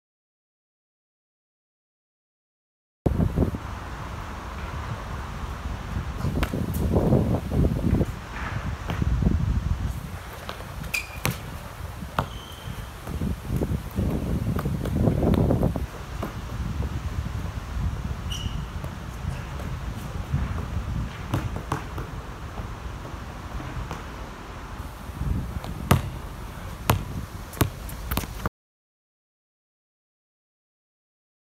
Mad Skills vs. Mad Skills - Freestyle Basketball Combo (July 30th)
MadSkillsTV Mad Skills vs. Mad Skills FreestyleBasketball Combo.
Testing my Brand New Shoes.